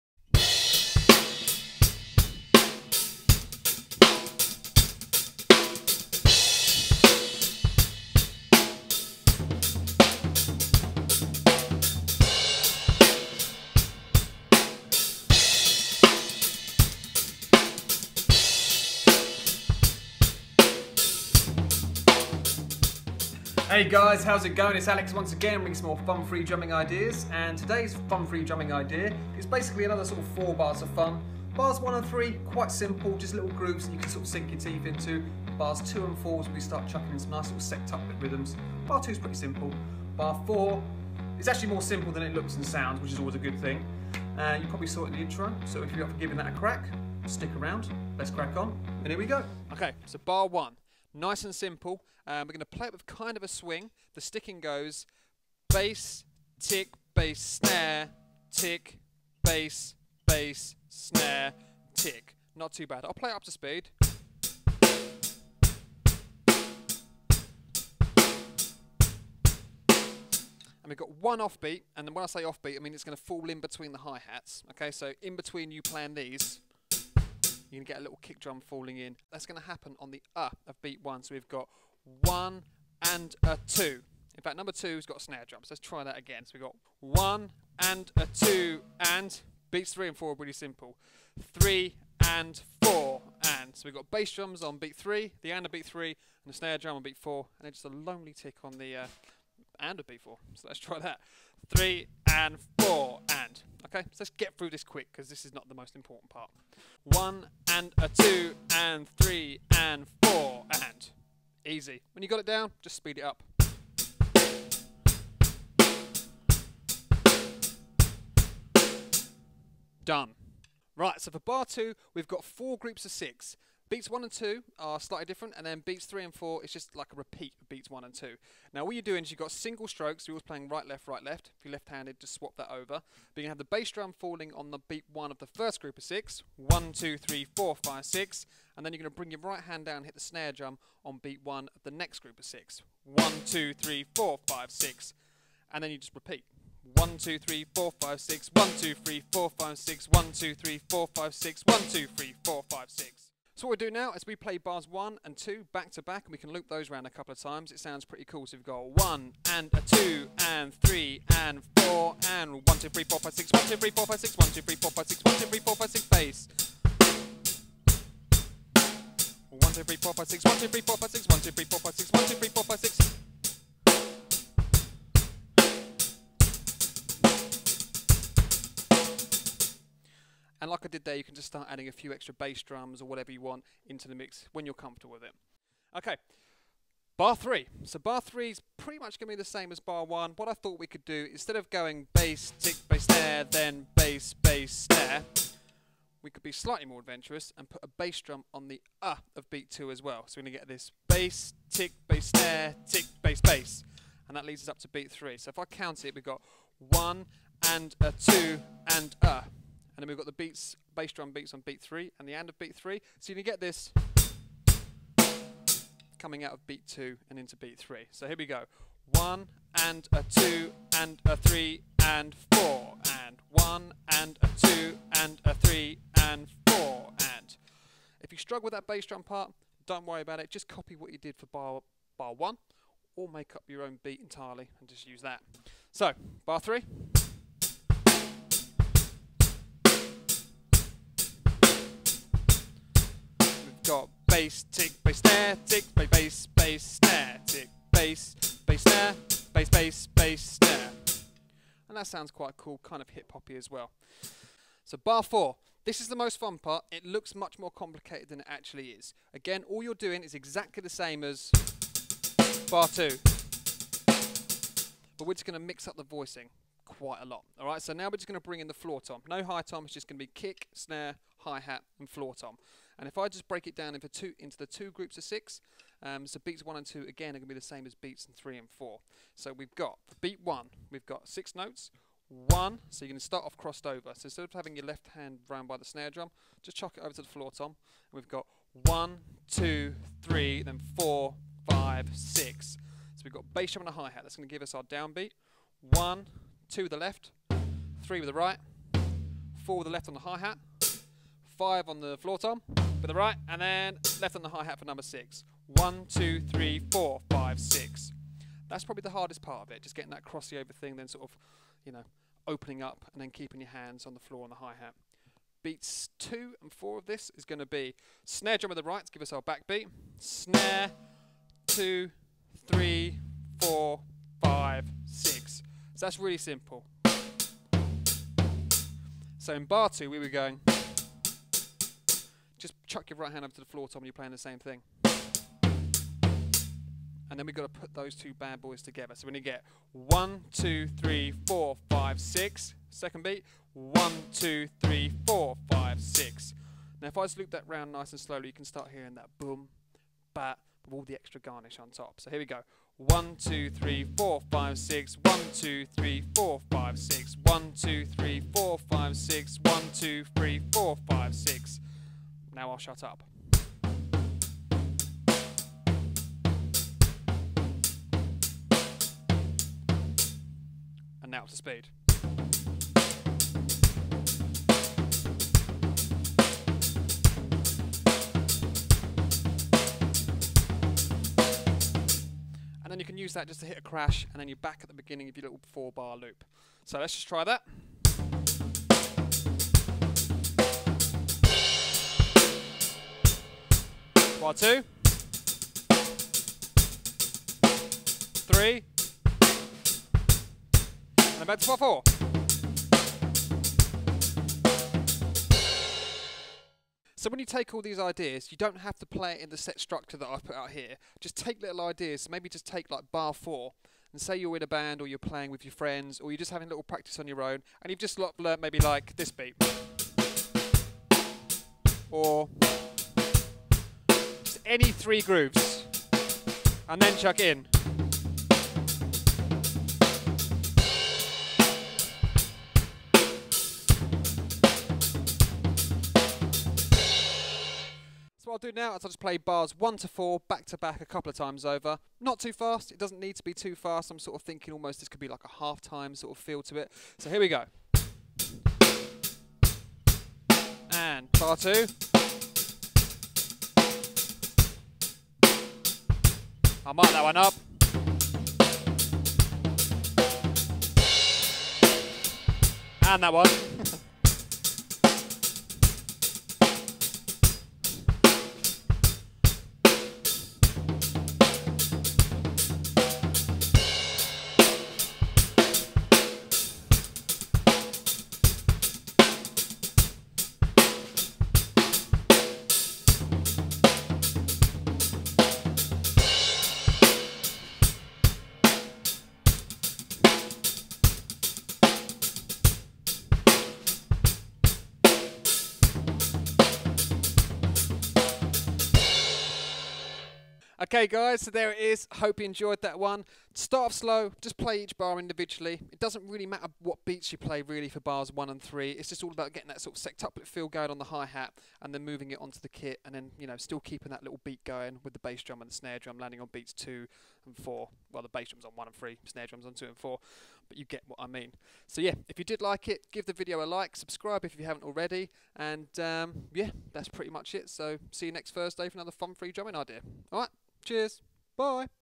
4 Bars Of Fun - Sextuplets - Drum Lesson #164
XBOX LIVE: RIBBIE

How to play the drums. In this weeks lesson we have got 4 new drum ideas to have a go at. Bars 2&4 are the real focus of the lesson, as we have a crack at playing sextuplet based grooves around the kit in a rock context. Enjoy!

You will also find links to my facebook page and twitter, as well as xbox live if you are a gamer like me. Cheers. Alex =)